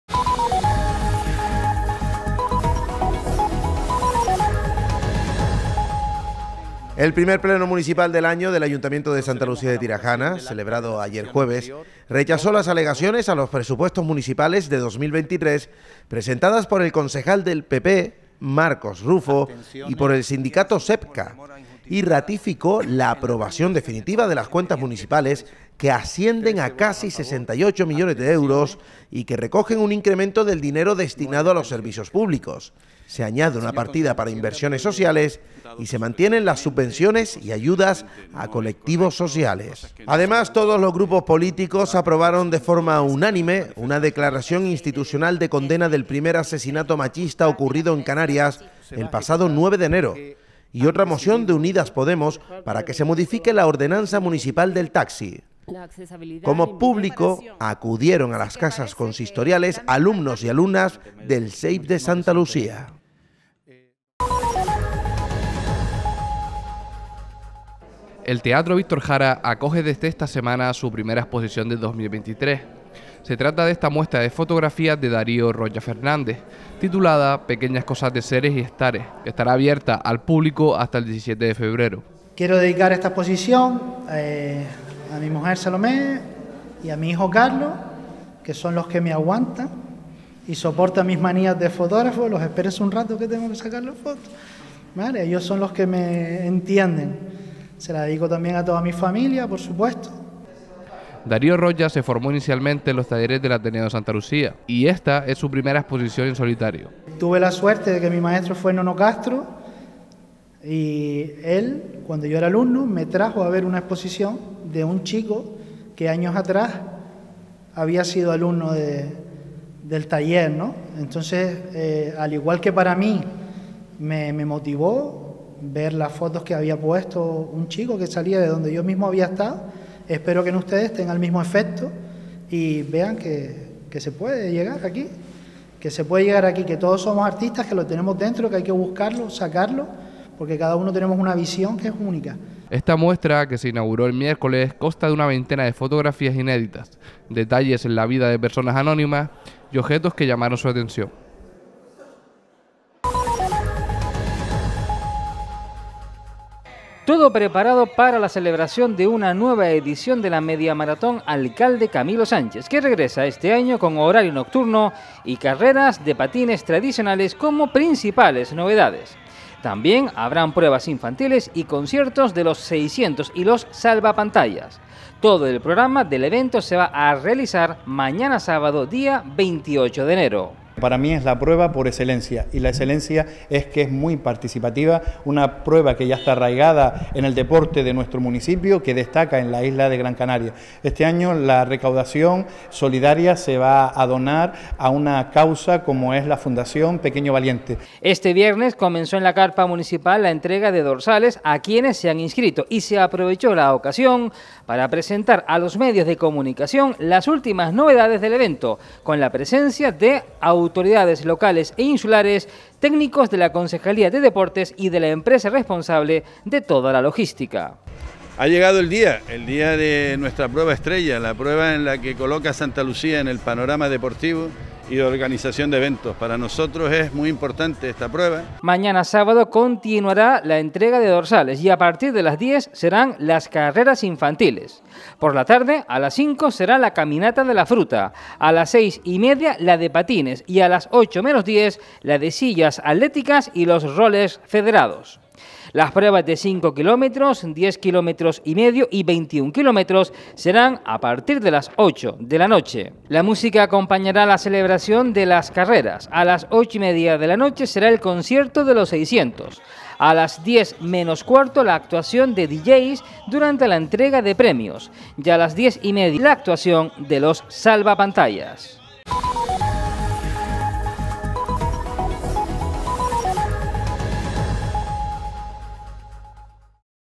INFORMATIVO SANTA LUCÍA WEB - 27 01 2023
Informativo del municipio de Santa Lucía:
1-Pleno ordinario del mes de enero de 2023.
2-Apertura de la exposición fotográfica "Cosas Pequeñas de Seres y Estares".
3-Entrega de dorsales de la Media Maratón Alcalde Camilo Sánchez.